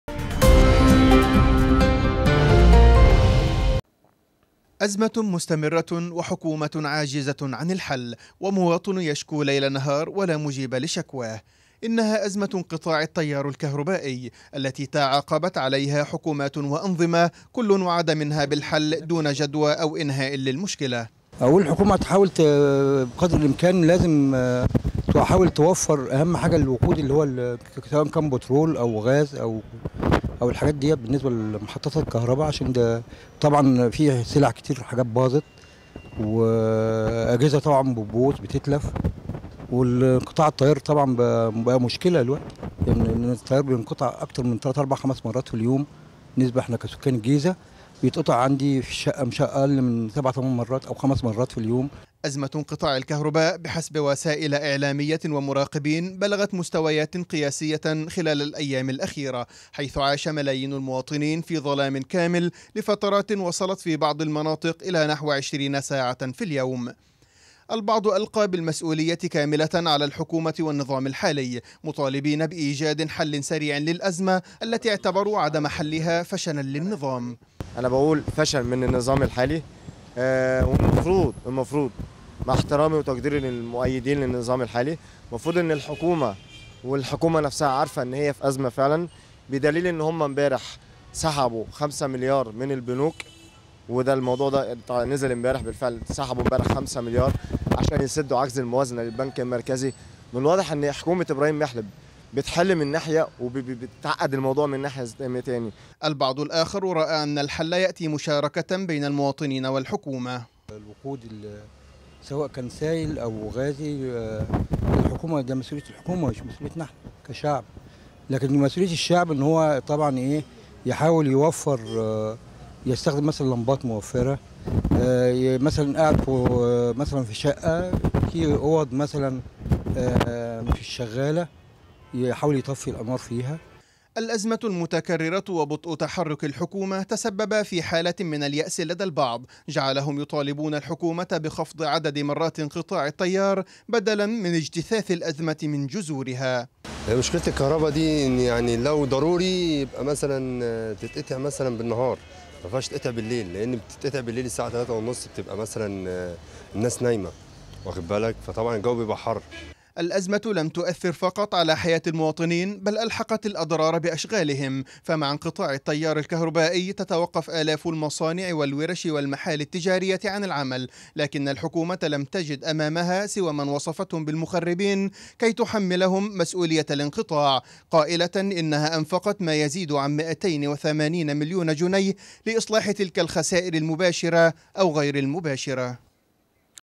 تفاقم معاناة انقطاع الكهرباء.. والانقطاع يصل لـ ٢٠ ساعة
تقرير عن معاناة انقطاع الكهرباء
متابعة إخبارية ١٩-٨-٢٠١٤م